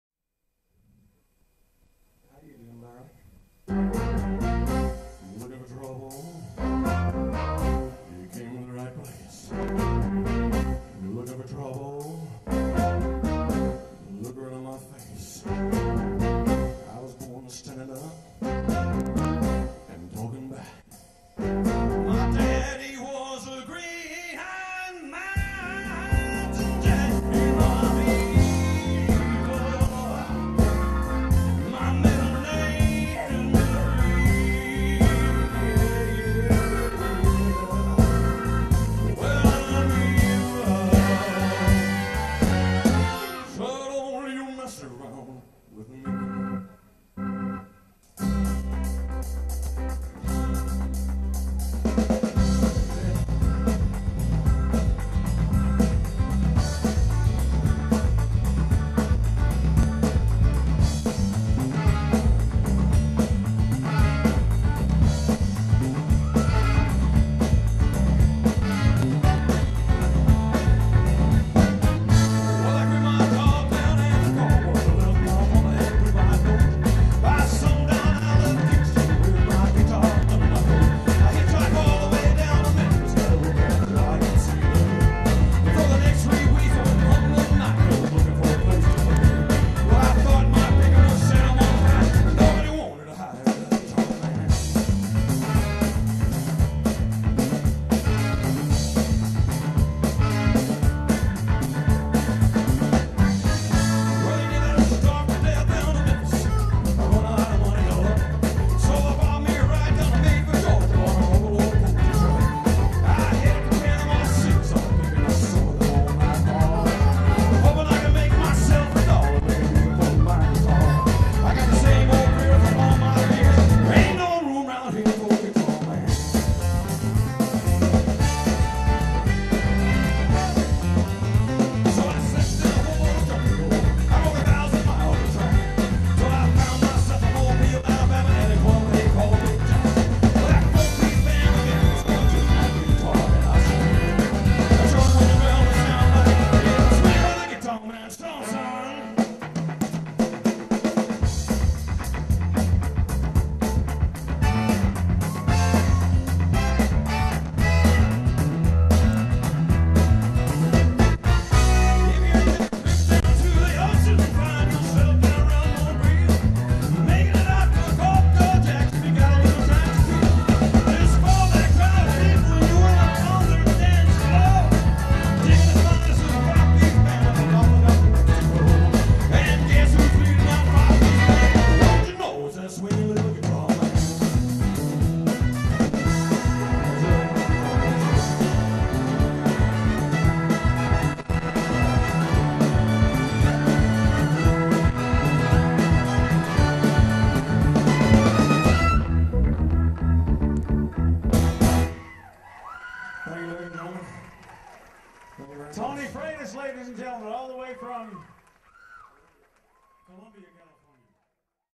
Tony Freitas as Elvis LIVE from Memphis Clip 5
This is Tony performing at the Images of the King competition in Memphis TN, August 2007.